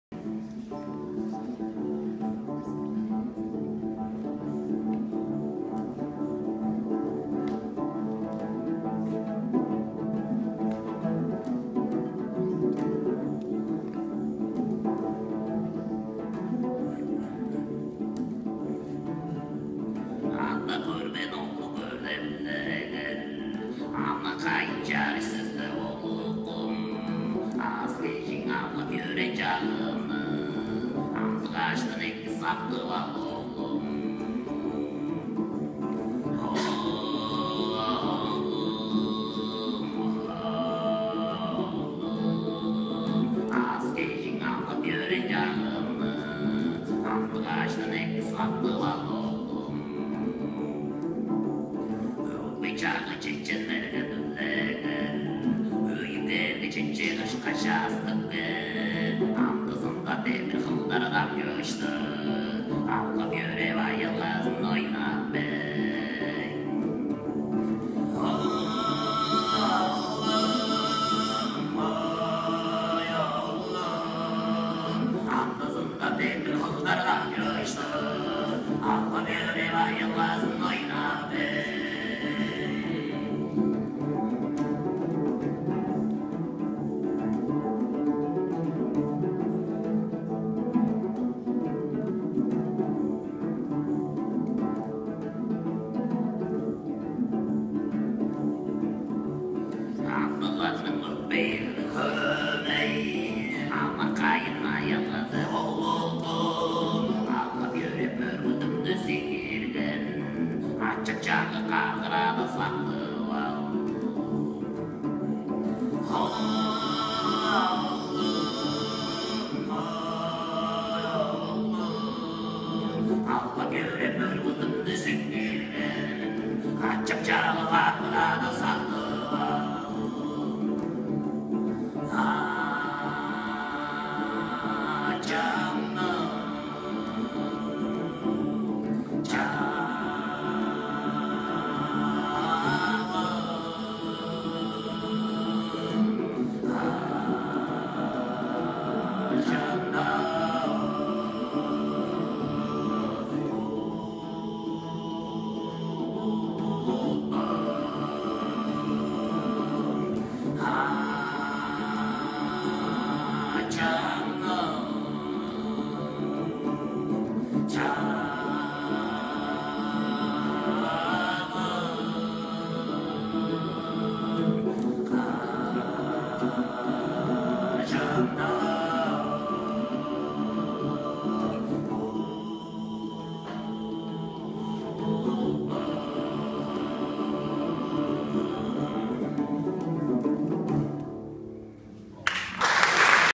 Alash Ensemble at WTAMU 4-22-14 Part 6 of 17
Alash Ensemble. Tuvan Throat Singers performing at West Texas A&M University 4-22-14 Part 6 of 17.